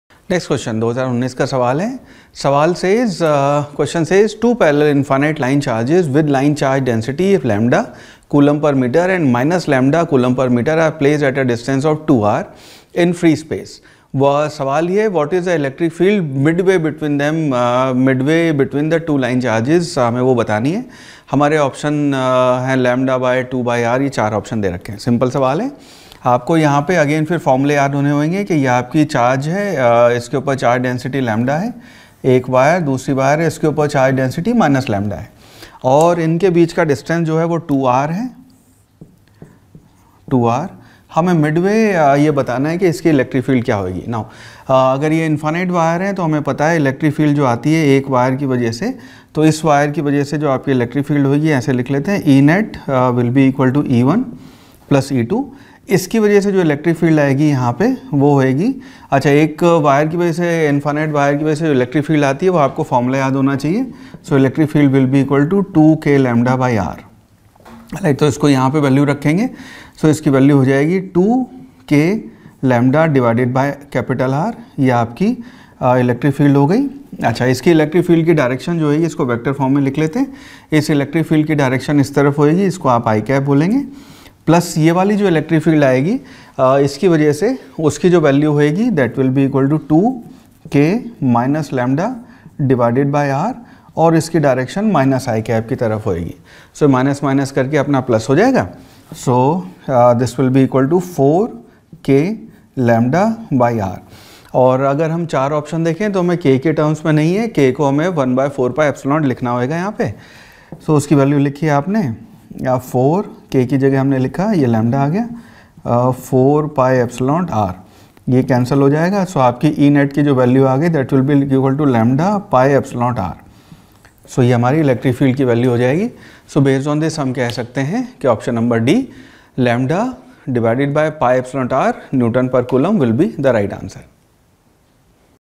Two parallel infinite line charges with linear charge densities are placed at a distance of 2 R in
Two parallel infinite line charges with linear charge densities are placed at a distance of 2 R in free space. What is the electric field mid-way between the two line charges